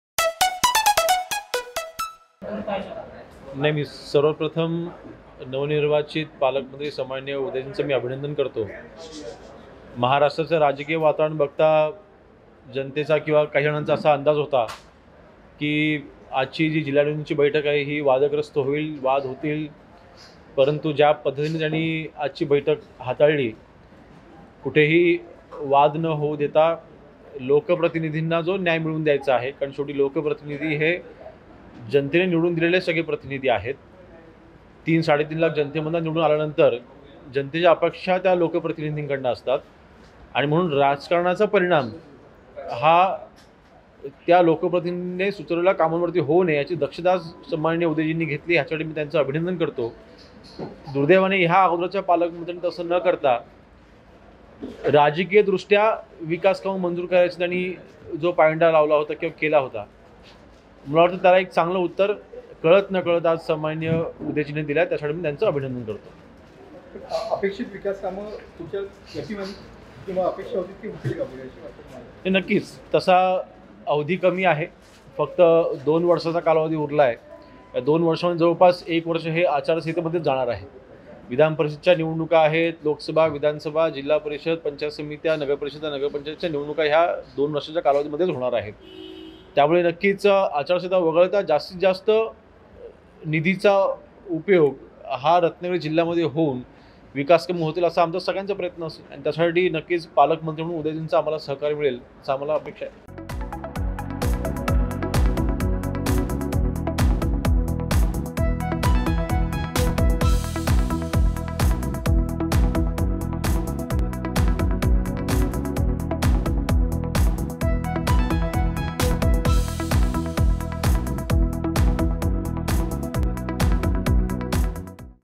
Yogesh Kadam यांनी Anil Parab यांना काय लगावला टोला | Uday Samant | Eknath Shinde
अगोदरच्या पालकमंत्र्यांनी राजकिय दृष्ट्या विकासकामं मंजूर करण्याचा पायंडा पाडला होता, त्याला आज चांगलं उत्तर कळत न कळत पालकमंत्री उदय सामंत यांनी आजच्या जिल्हा नियोजन समितीच्या बैठकीतून दिलं आहे, असं म्हणत आमदार योगेश कदम यांनी माजी पालकमंत्री अनिल परब यांच्यावर निशाणा साधला आहे.